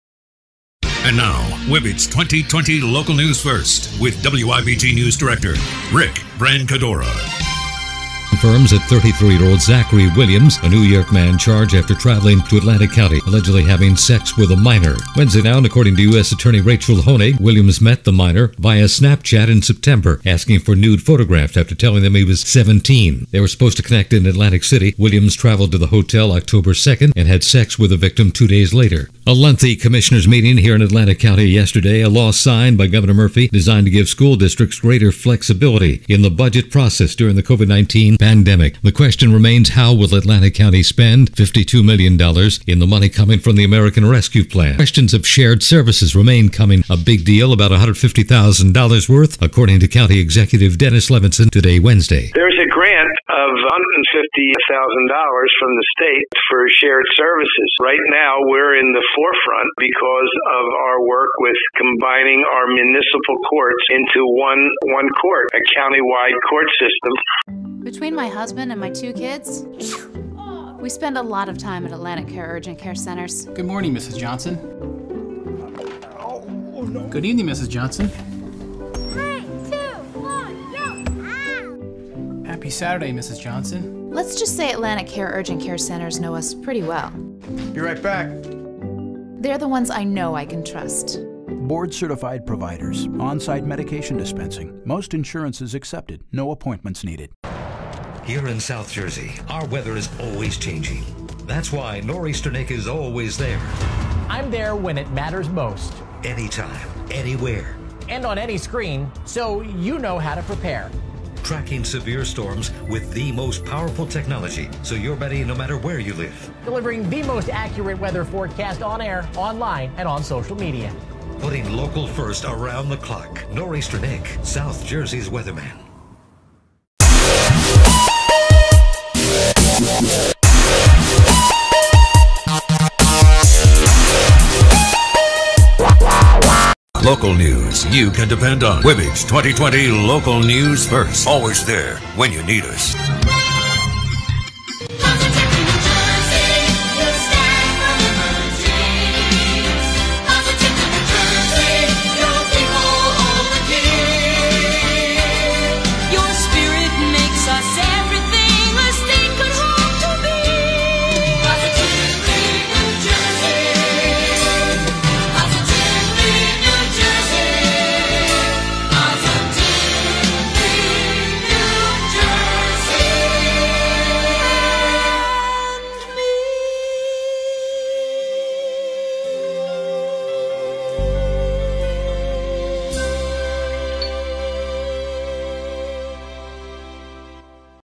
THURSDAY WIBBAGE SOUTH JERSEY MORNING NEWS BRIEF
WIBG Wibbage 94.3 South Jersey News Brief Thursday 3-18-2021